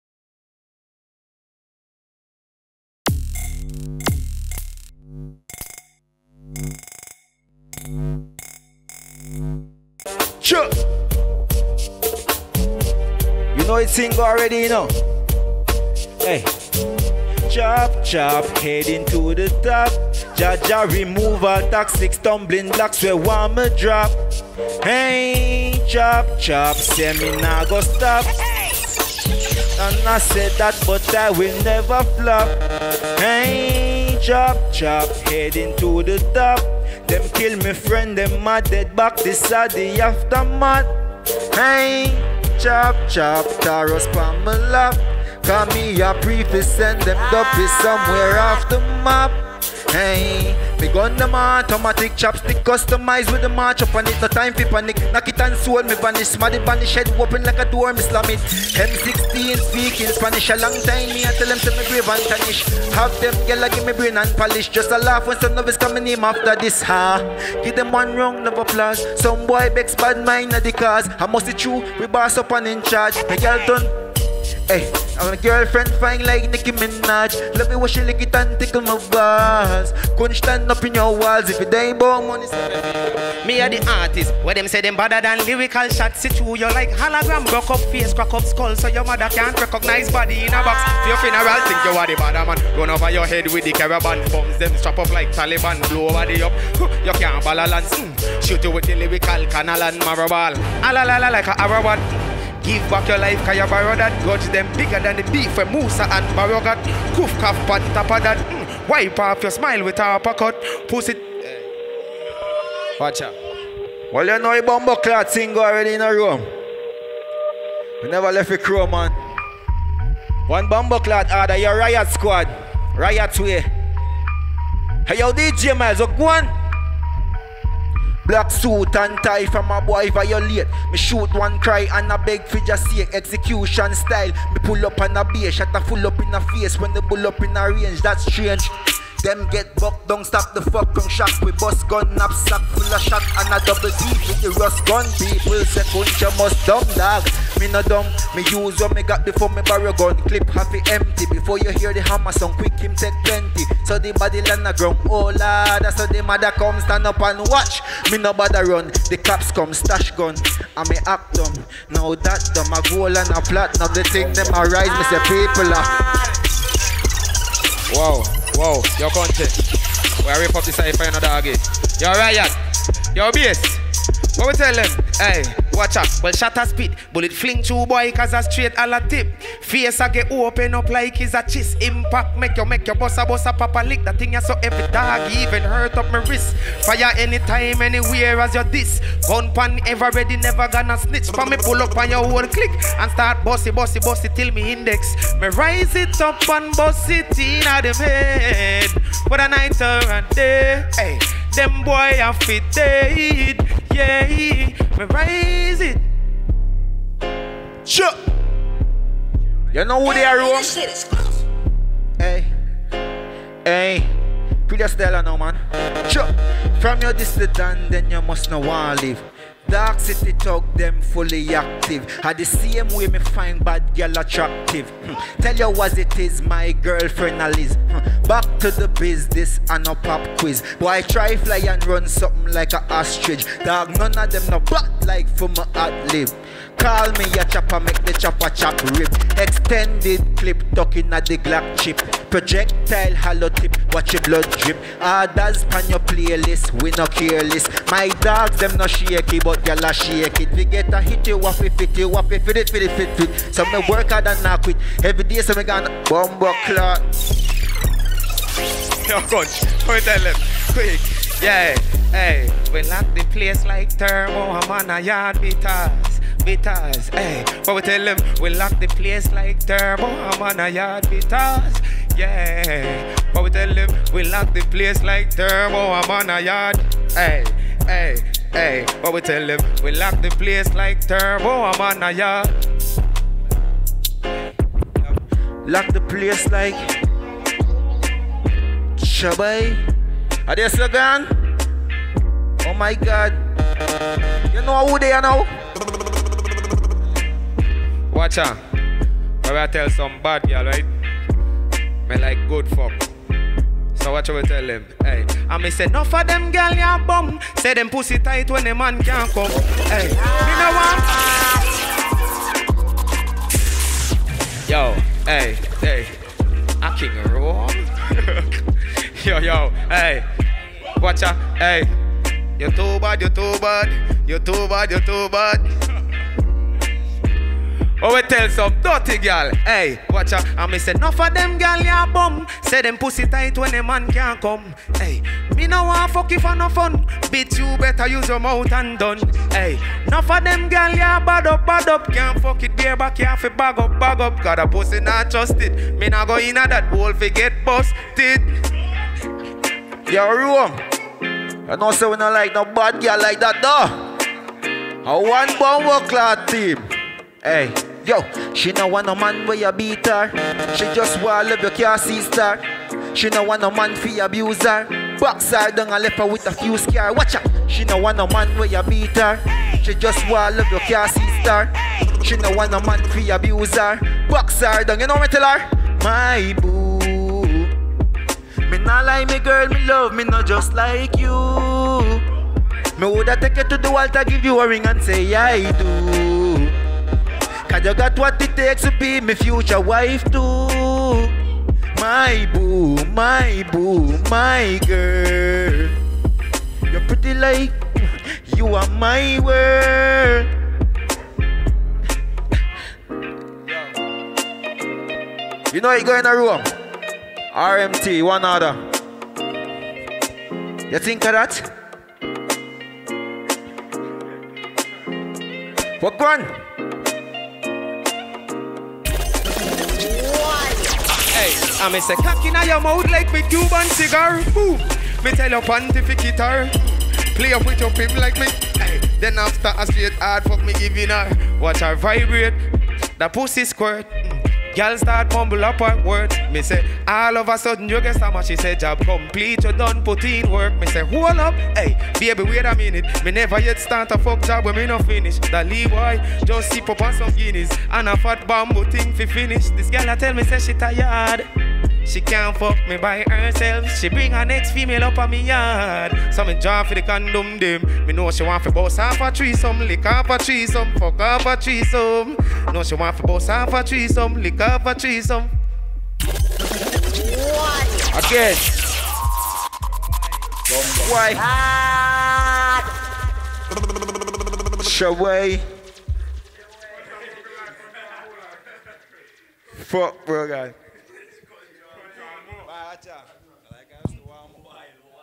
RIOT SQUAD FT KING ROME , KUNCHY - Cypher | Di Top Live Show
Today we team up with our good friends @Riot Squad Entertainment to bring to you something a little different, along with a brand new artist from the Gales Point Manatee @kunchy1aada also fan favorite @kingrome_official